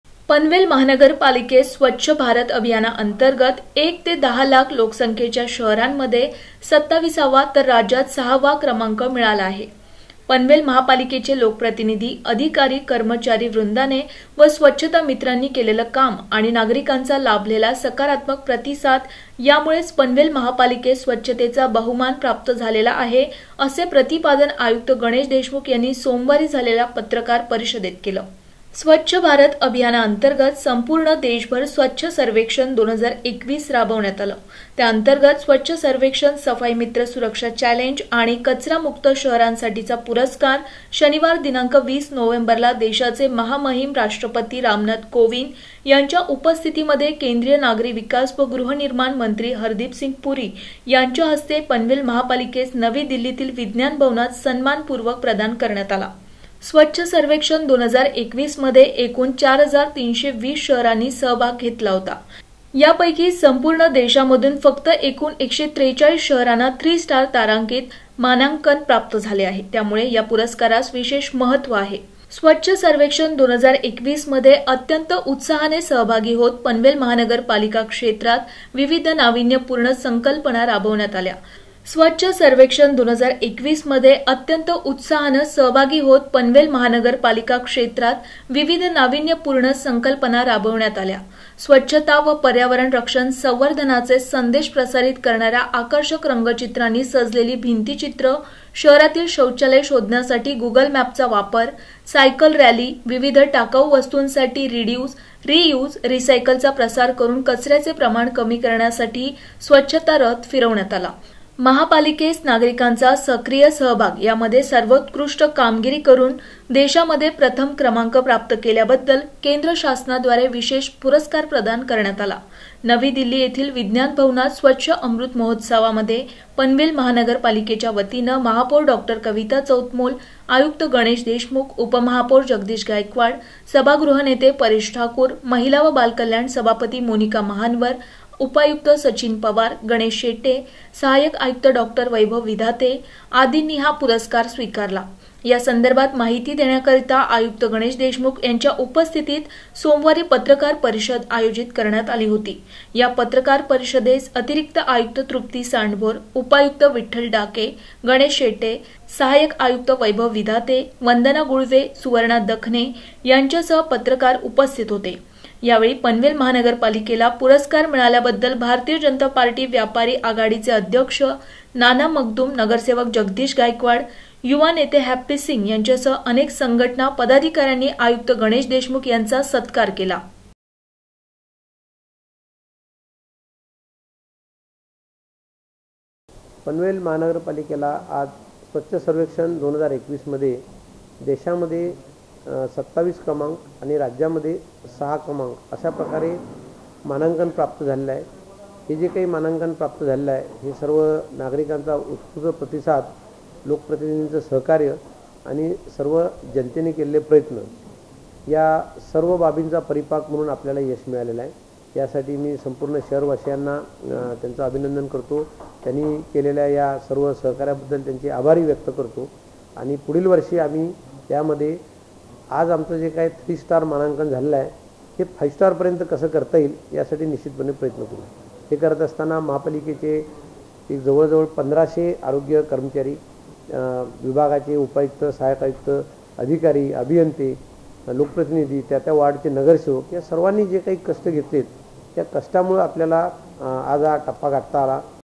पनवेल महापालिकेचा दिल्लीत गौरव;आयुक्त गणेश देशमुख यांनी पत्रकार परिषद घेऊन दिली अधिक माहिती !
पनवेल महापालिकेचा दिल्लीत गौरव;
१ ते १० लाख लोकसंख्येच्या शहरांमध्ये देशांमध्ये २७ वी रॅंक मिळाली, तर राज्यामध्ये २ री रॅंक;
आयुक्त गणेश देशमुख यांनी पत्रकार परिषद घेऊन दिली अधिक माहिती !